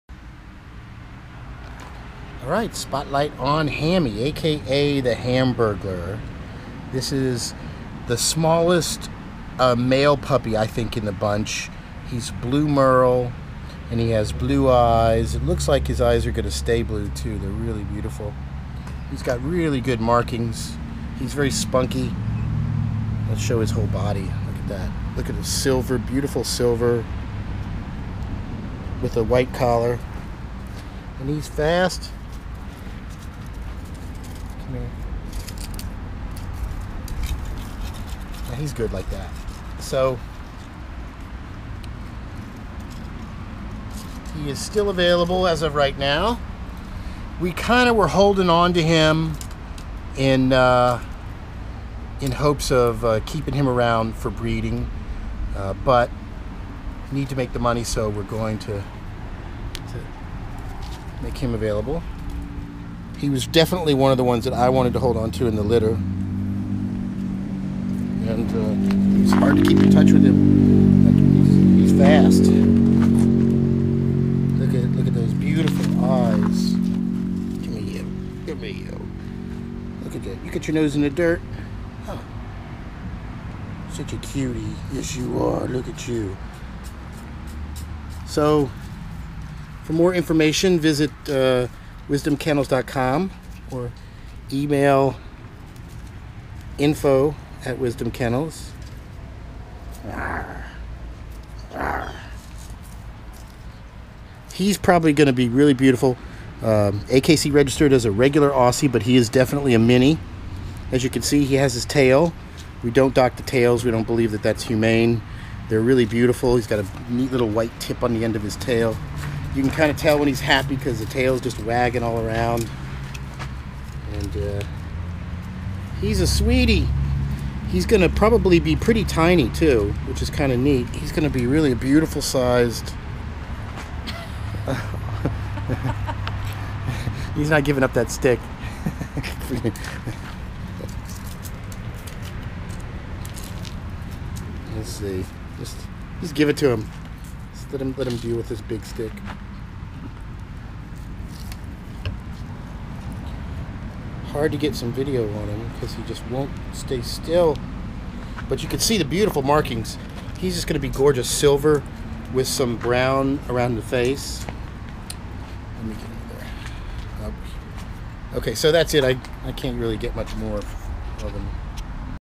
Puppy #5 "Hammy" Blue Merle male w/blue eyes - WisdomKennels.com
Still available - gorgeous blue merle male mini-aussie (AKC registered as Australian Shepherd) puppy - for details - available in the New Orleans, LA area